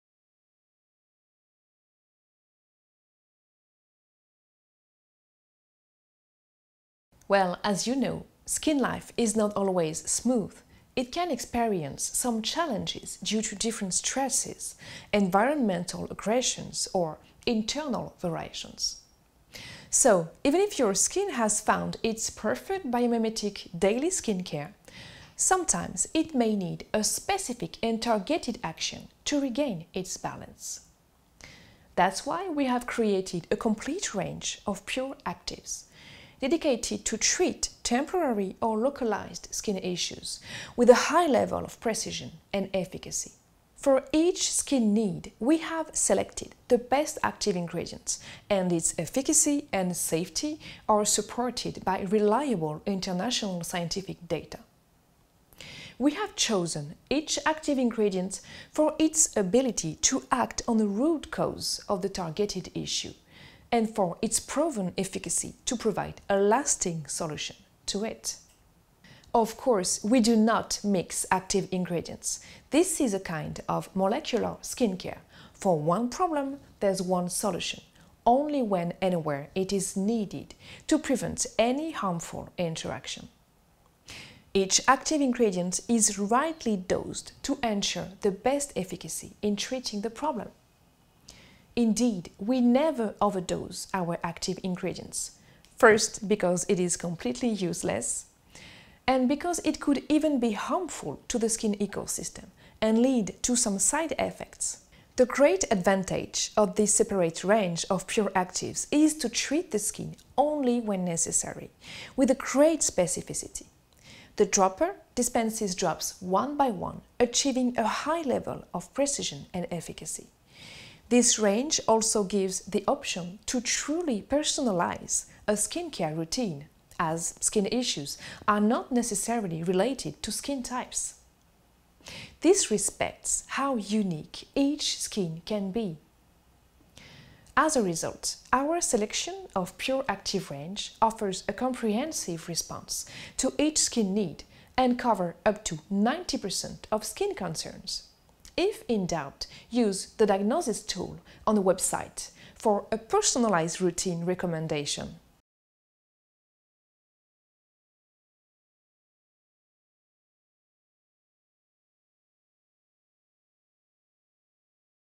Какво са Чистите активни молекули на Etat Pur?
Елоиз Коле, мениджър "Научни комуникации" на NAOS, обяснява какво представлява линията "Чисти активни молекули" на Etat Pur и каква роля имат те в програмата за грижа за кожата.

Точно това, от което се нуждае кожата Ви. Нито повече, нито по-малко.